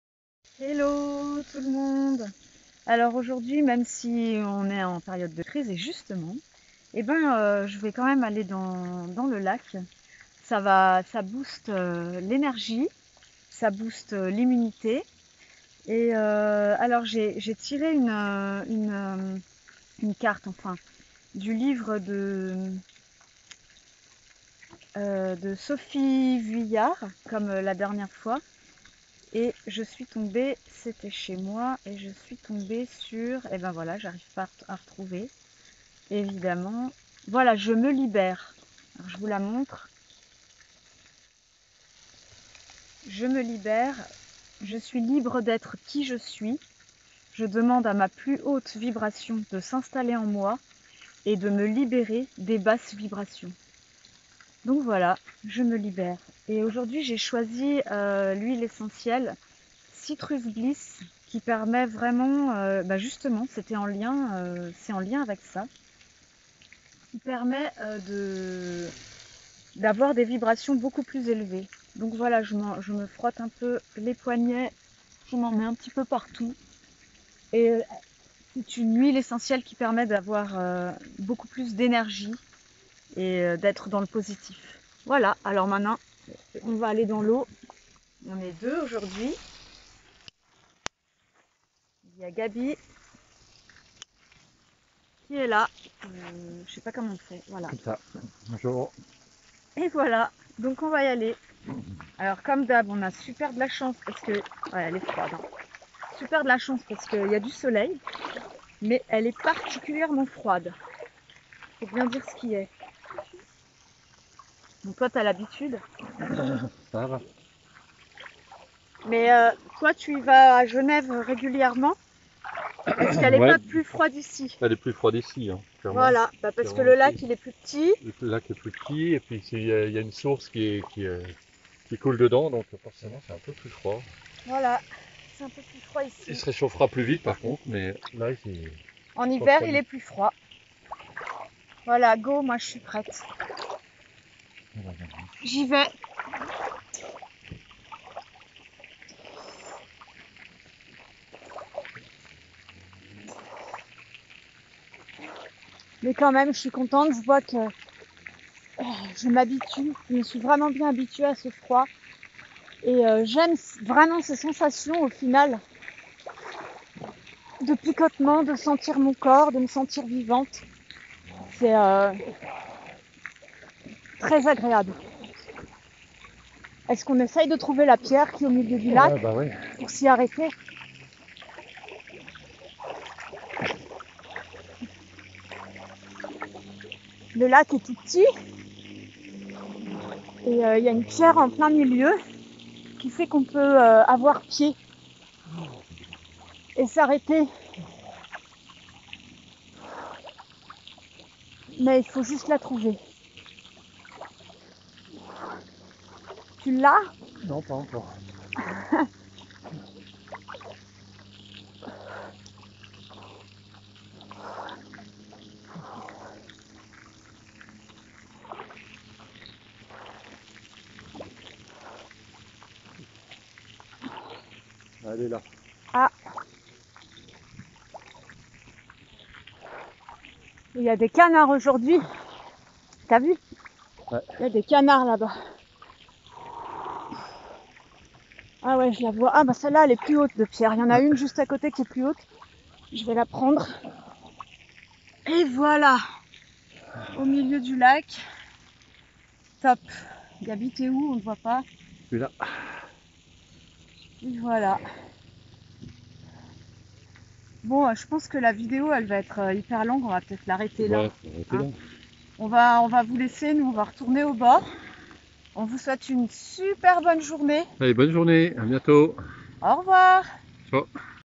Défi baignade au lac le 15 mars 2020
Élever ses vibrations avec le message du livre de Sophie Vulliard et Citrus Bliss de DoTerra